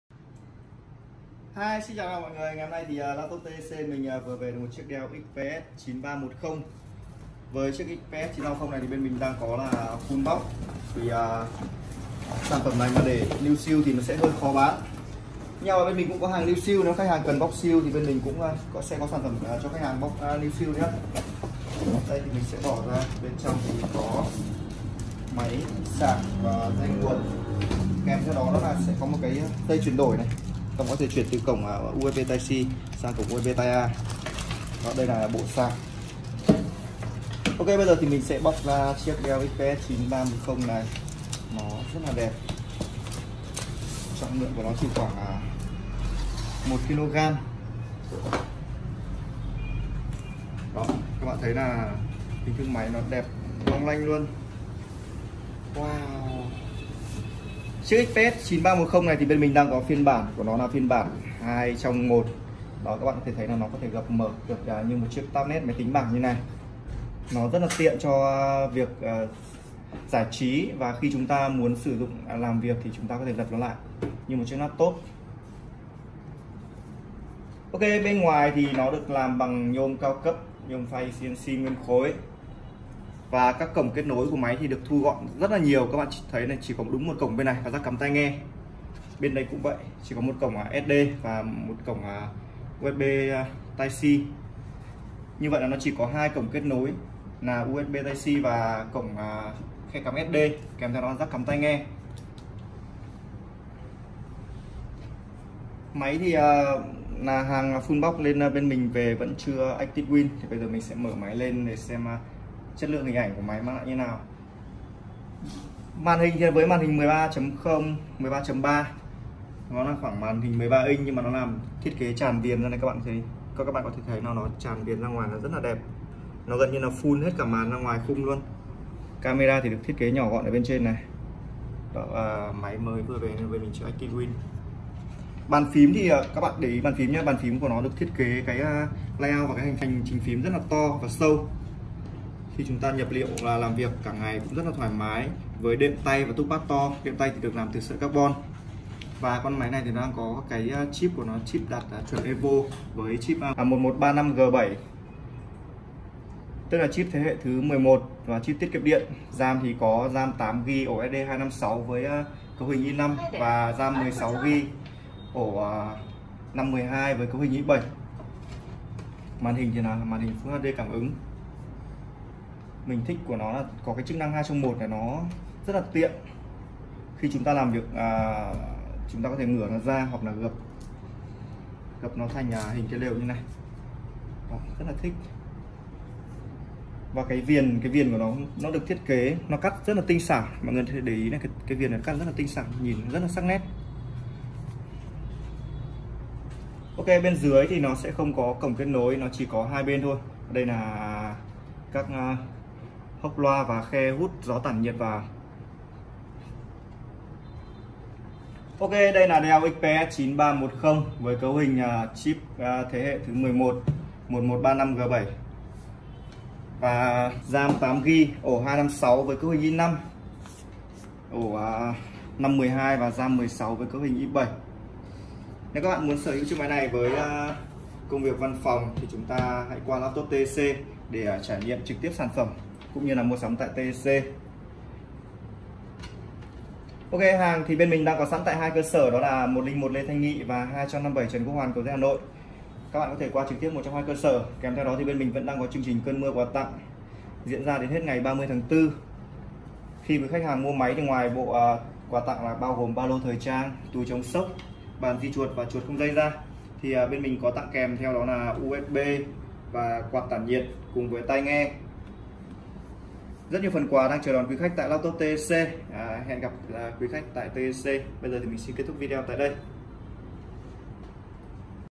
SIÊU PHẨM NĂM 2021: Dell XPS 9310 mẫu laptop sang trọng, mỏng nhẹ của năm 2021
Nếu các bạn đang muốn sử hữu 1 mẫu laptop mỏng nhẹ, sang trọng cấu hình cao thì đây sẽ là sự lựa chọn đúng đắn